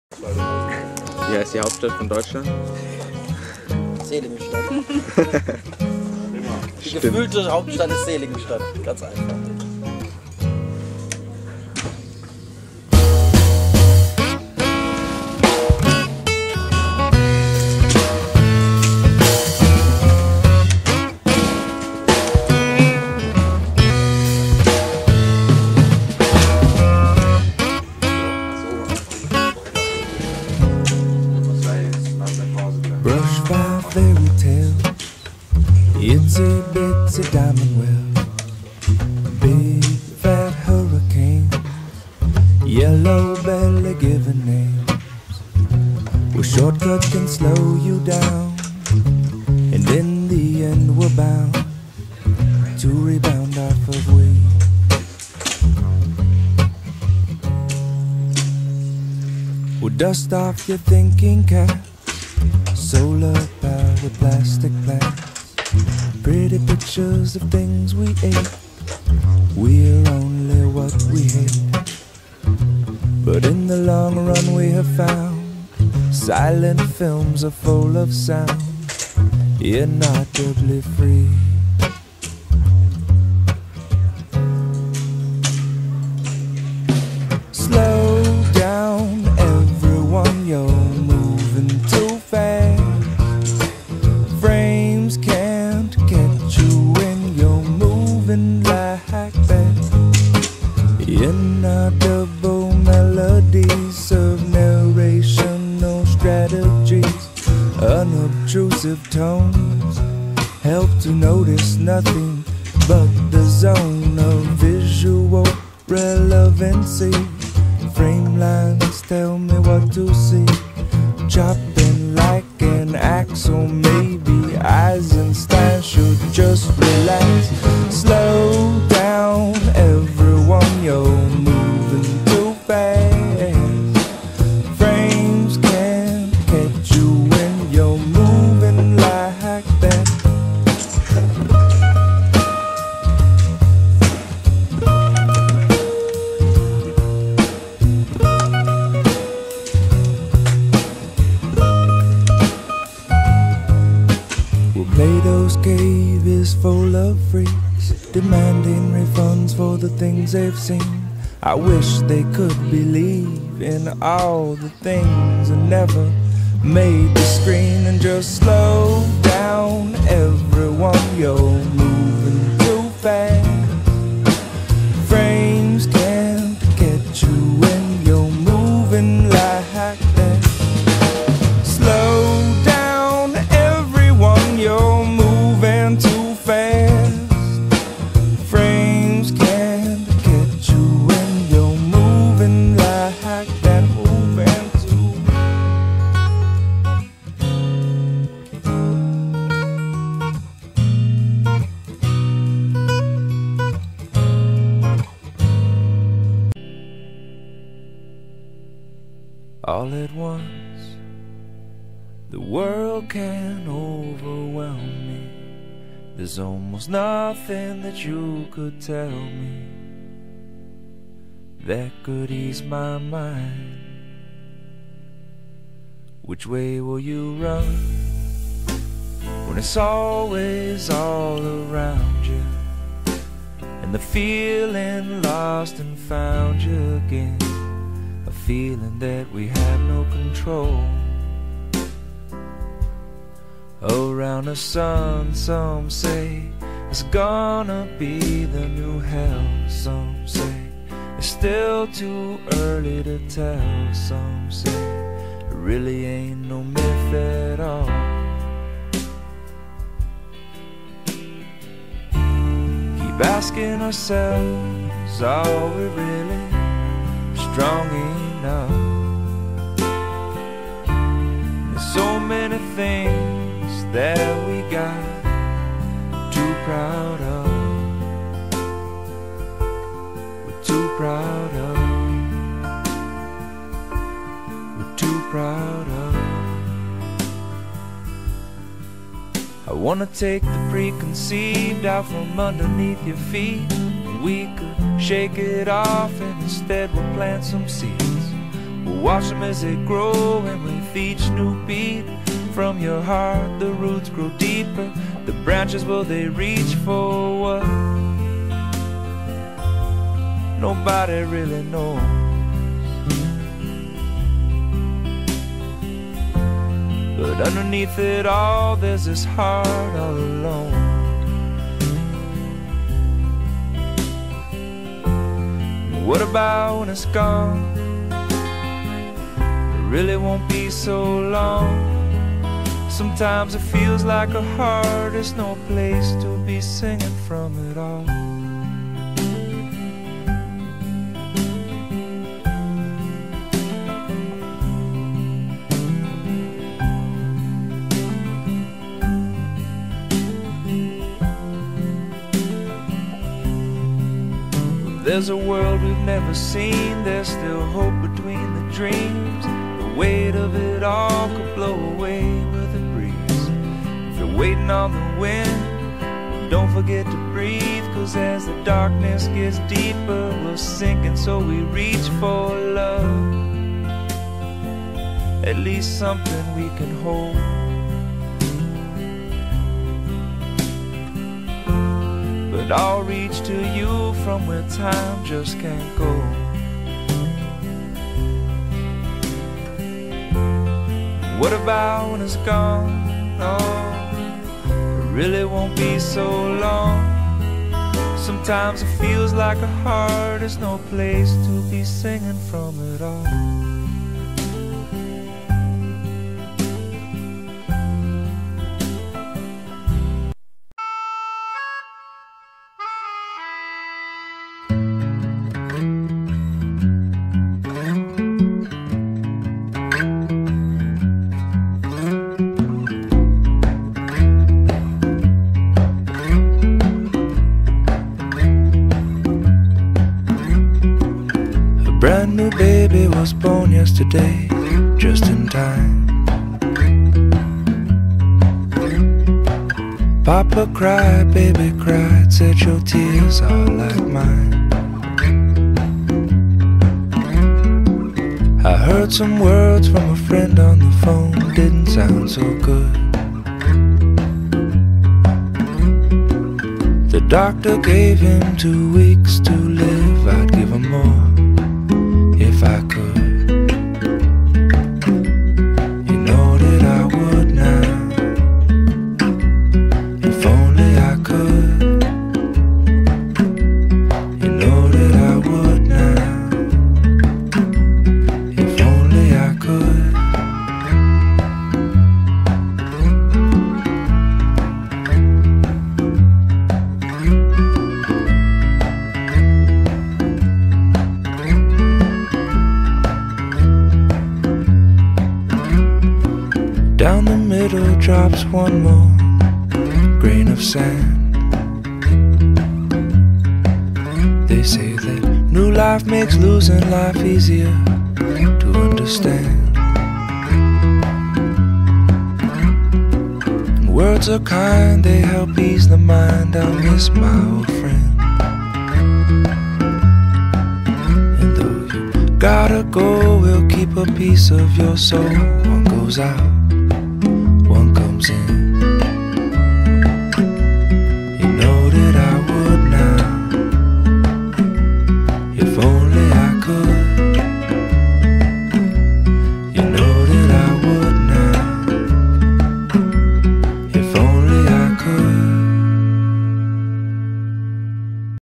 Gau Meisterschaft 2008
Ein kleiner Auszug aus dem recht langen Aufnahmen der beiden Tage in Seligenstad.

Weiteres wird folgen aber ich muss erst mal alles anschauen und weiter zusammenschneiden.